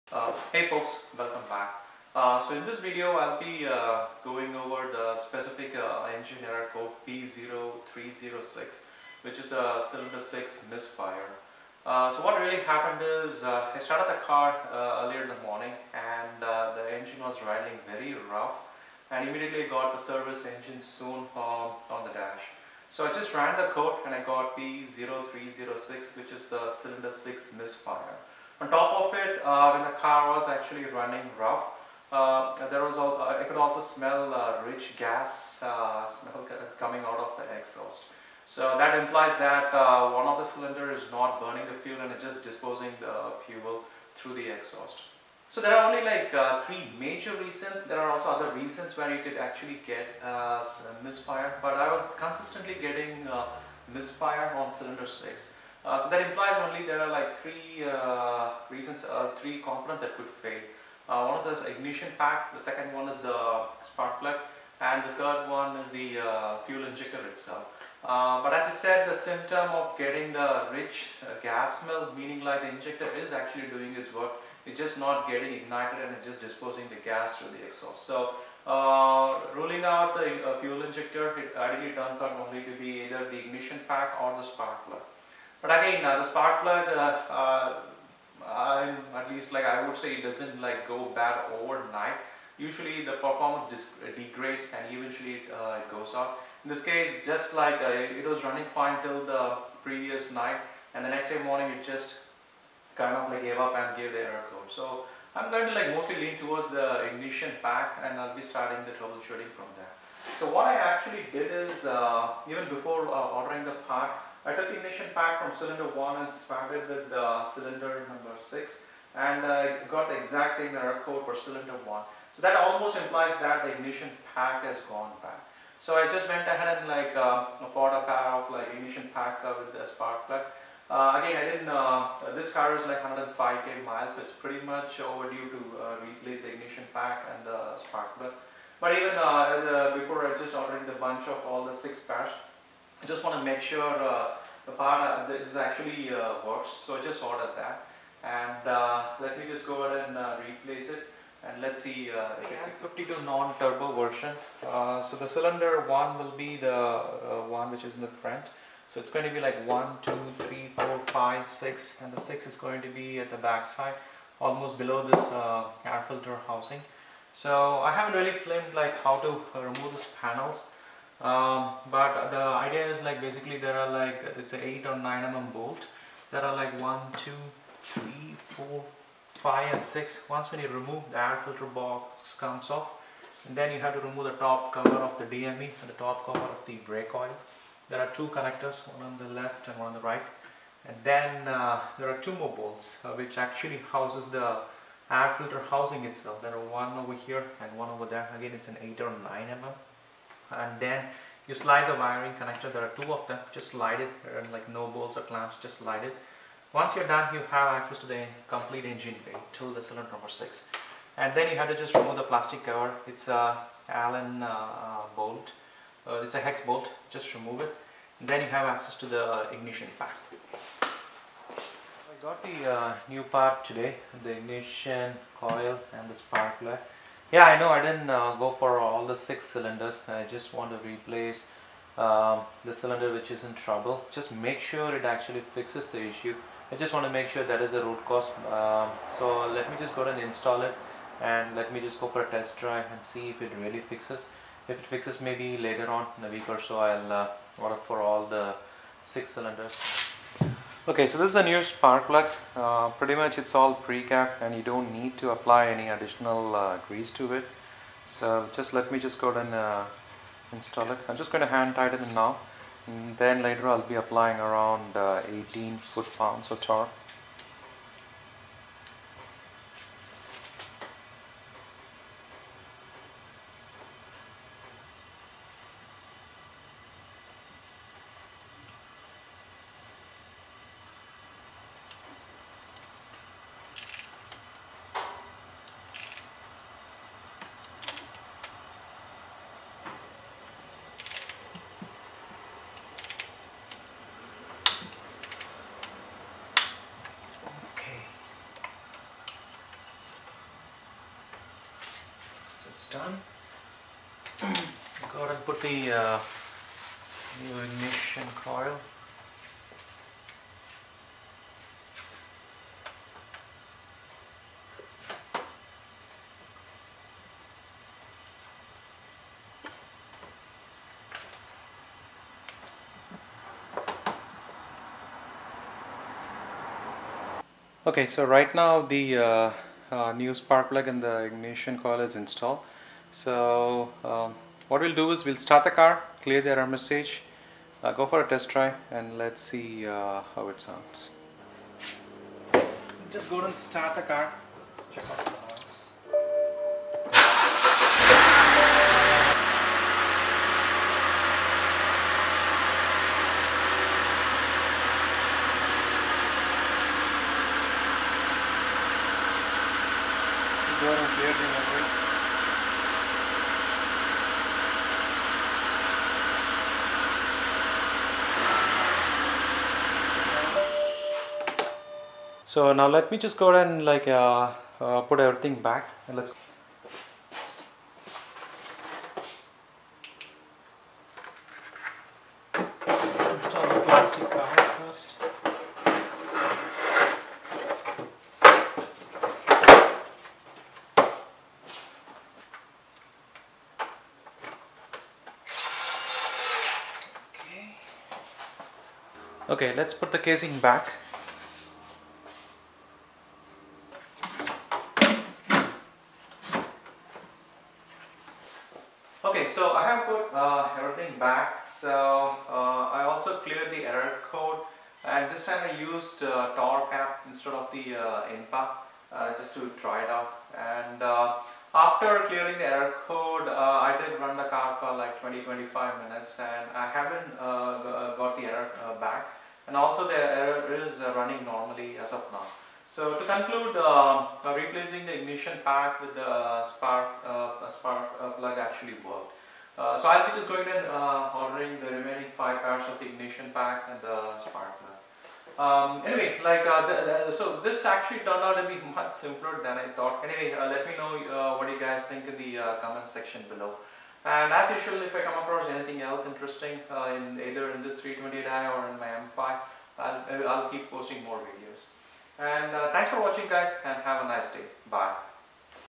BMW 328i - SES Error code P0306, P030X - Cylinder Misfire
Got an Engine Error code - P0306 [Cylinder 6 Misfire] and will be sharing my experience on resolving it. Other symptoms were rough idling, rich gas smell from exhaust pipe. Swapped the Ignition coil from Cylinder 6 to Cylinder 1 and got the error code P0301[Cylinder 1 Misfire]. So ended up replacing with new Ignition coil/Spark Plug.

Video BreakDown :-
0:00  Intro and Diagnosis
2:33  Instruction to remove plastic panels for Air Filter/Engine cover
3:42  Replace with new Spark Plug and Ignition Coil
5:41  Clear Error Code using Torque Android app
6:48  Conclusion

Parts replaced:-
Ignition Coil -  MFG part# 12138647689
Spark Plug - MFG part# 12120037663